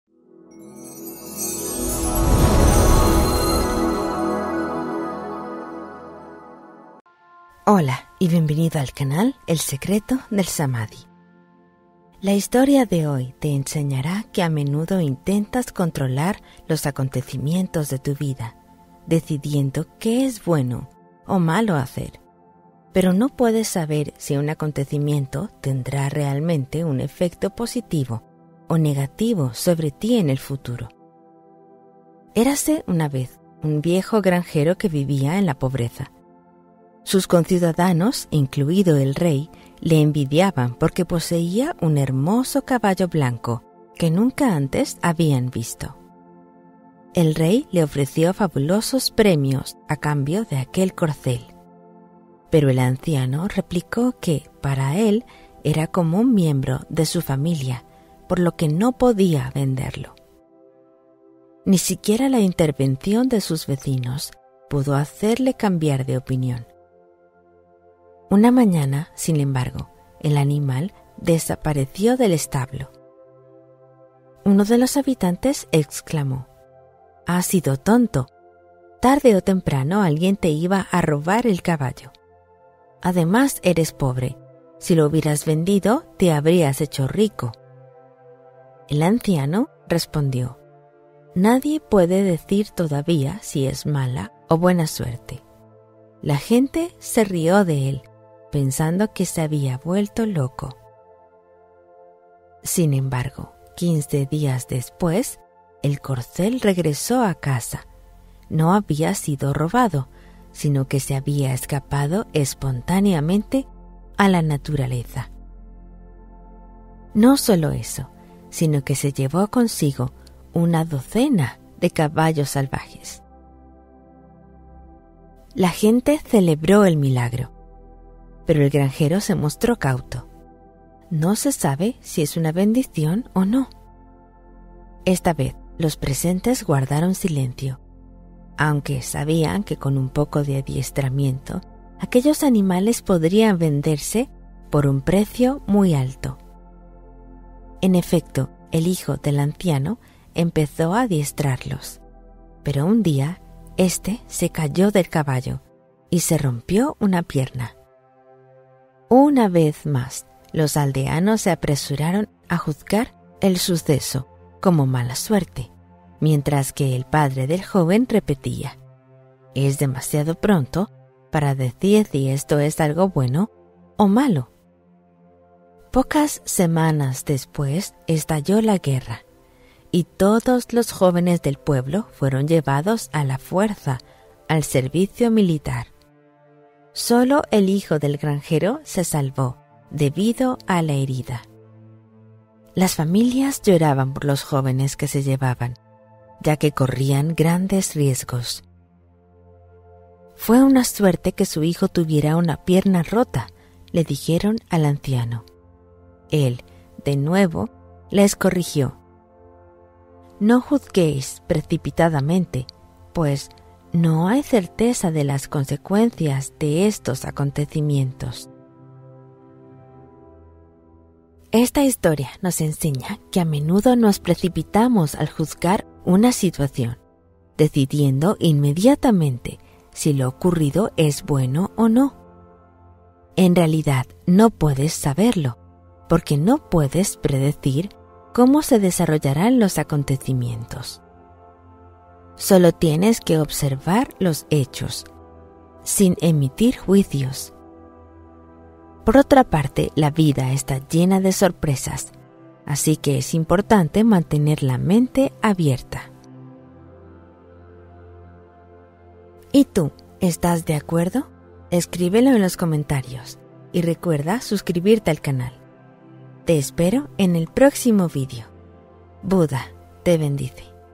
DE LA DESGRACIA A LA FORTUNA (HISTORIA BUDISTA DE LA DANZA INVISIBLE DEL DESTINO)
La historia nos enseña a no juzgar rápidamente los sucesos de la vida como buenos o malos, ya que sus consecuencias son inciertas. Un granjero pierde y recupera su caballo, adquiere más y su hijo se lesiona.

Sin embargo, la herida evita que el hijo vaya a la guerra, salvándole de los peligros. La lección es observar los hechos sin emitir juicios y mantener la mente abierta ante las sorpresas de la vida.